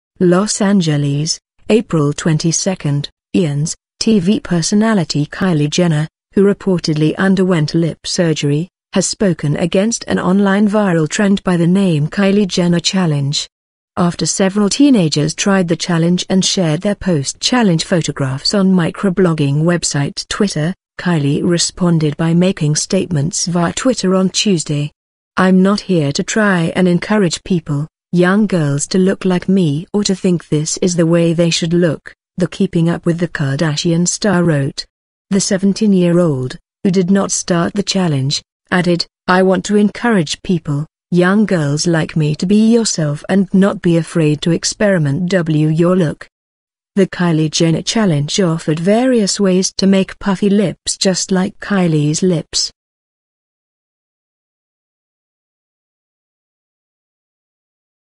KYLIE JENNER RESPONDS TO PUFFY LIP CHALLENGE
Los Angeles, April 22 (IANS) TV personality Kylie Jenner, who reportedly underwent lip surgery, has spoken against an online viral trend by the name 'Kylie Jenner Challenge'.

After several teenagers tried the challenge and shared their post-challenge photographs on micro-blogging website Twitter, Kylie responded by making statements via Twitter on Tuesday.

"I'm not here to try & encourage people/young girls to look like me or to think this is the way they should look," the "Keeping Up with the Kardashian" star wrote.

The 17-year-old, who did not start the challenge, added: "I want to encourage people/young girls like me to be YOURSELF & not be afraid to experiment w your look."

The "Kylie Jenner Challenge" offered various ways to make puffy lips just like Kylie's lips.